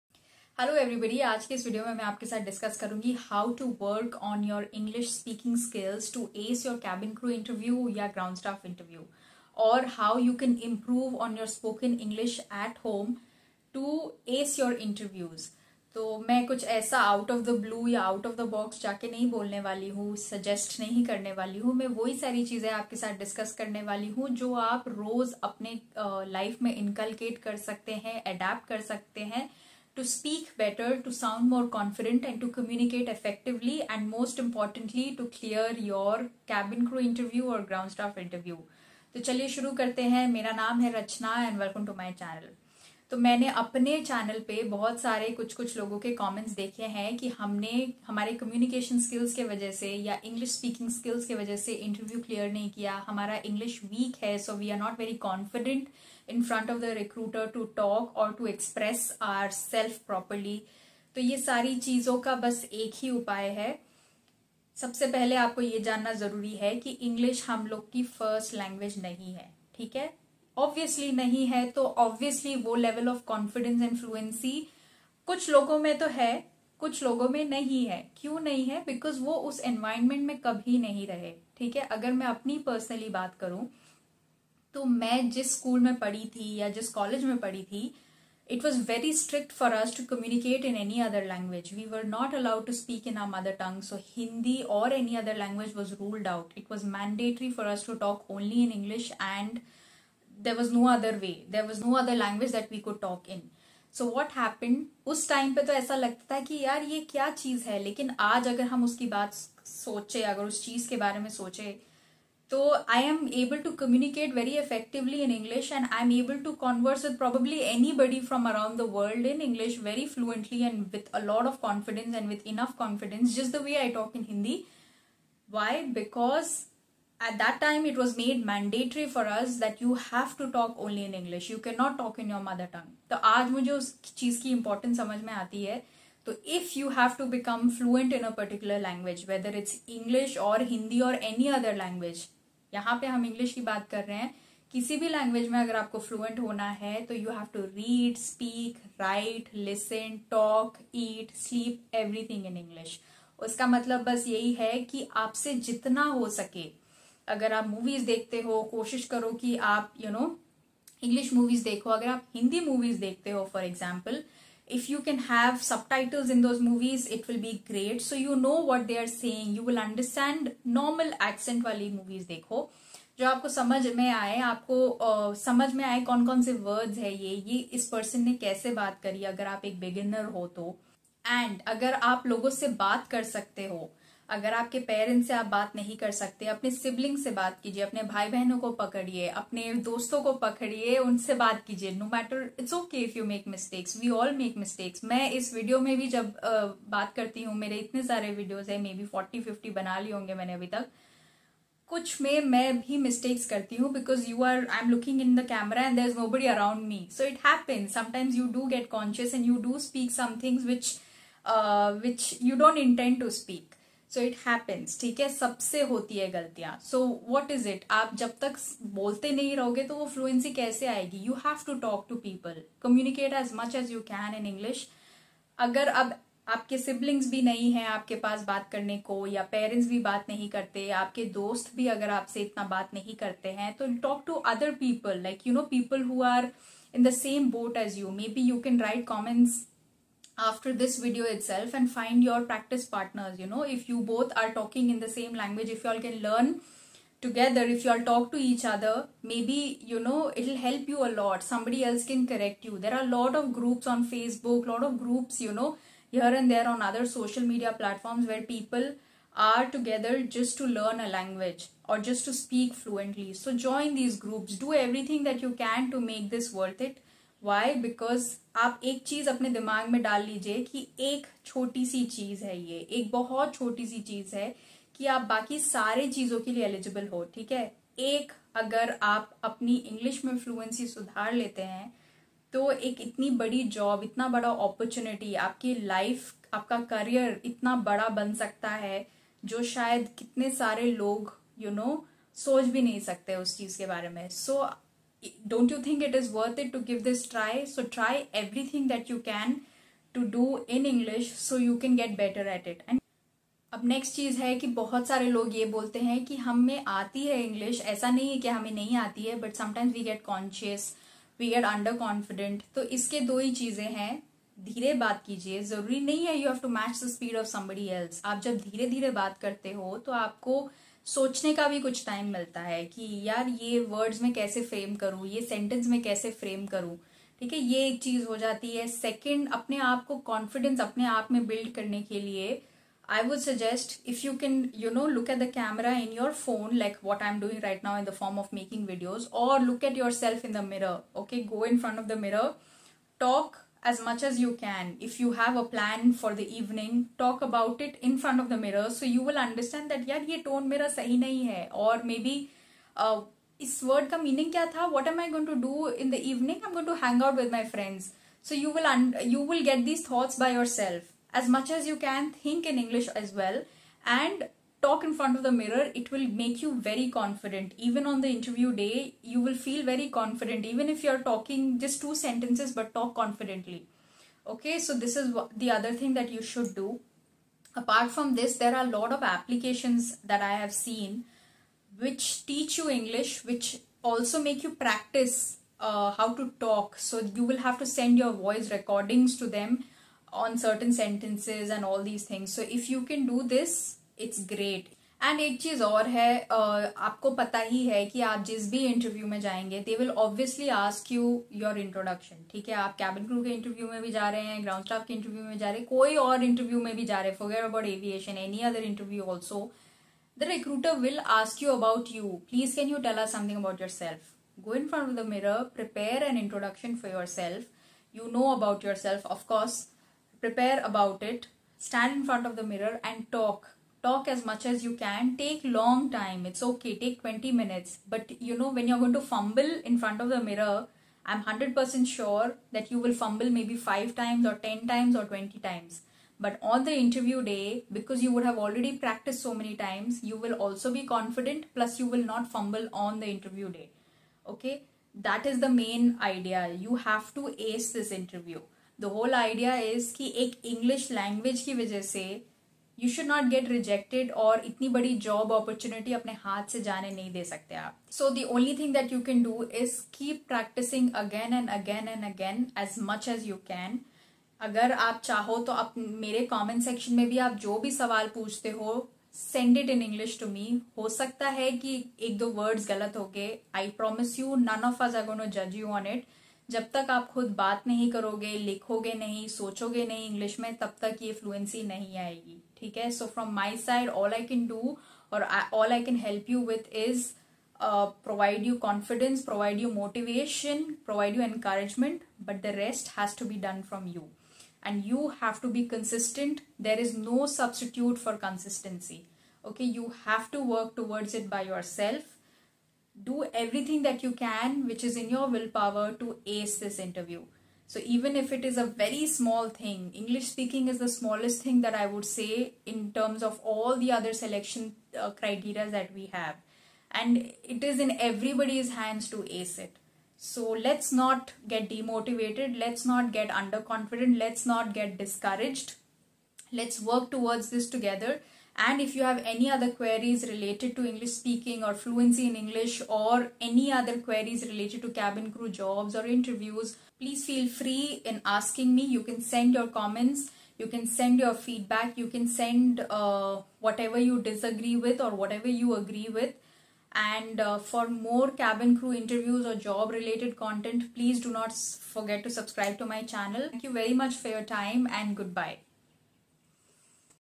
How to work on your English to become a cabin crew or groundstaff ? Tips to become fluent in english
How to work on your English to become a cabin crew or groundstaff ?
Ace your cabin crew / groundstaff interview

In this video I have discussed about how you can improve on your english fluency and ace your cabin crew or ground staff interview .

Thanks .

Tags- work on your english to ace your cabin crew interview,
how to speak english fluently and confidently for cabin crew,